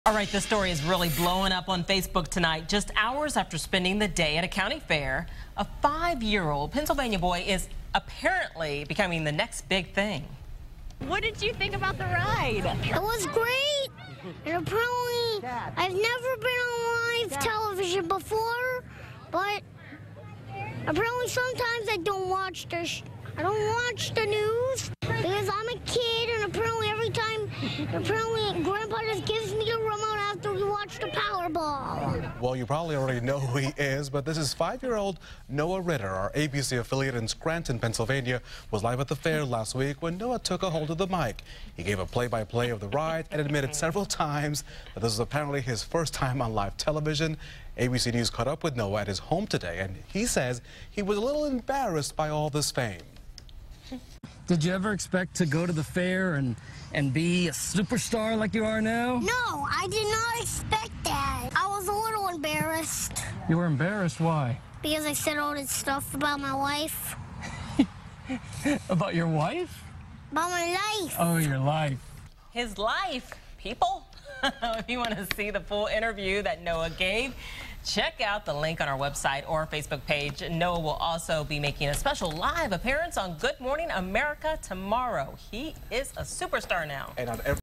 5-year-old boy is 'apparently' famous
Hours after spending the day at a county fair, a 5-year-old Pennsylvania boy is "apparently" becoming the next big thing.